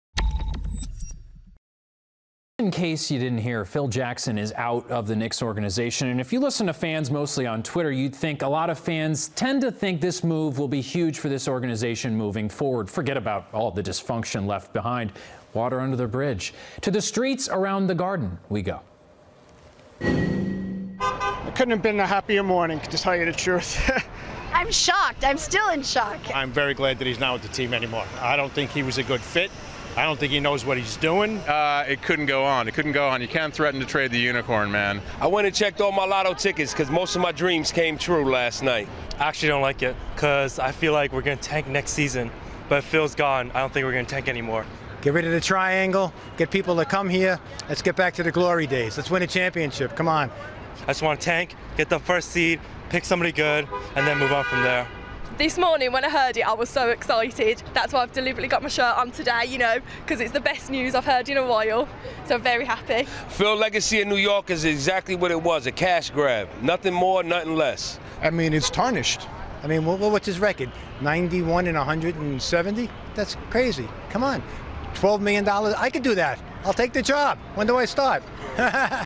Knicks fans outside of MSG share takes on Phil Jackson firing
New York Knicks fans outside of Madison Square Garden give their takes on the dismissal of Phil Jackson and his failed tenure as head of the Knicks.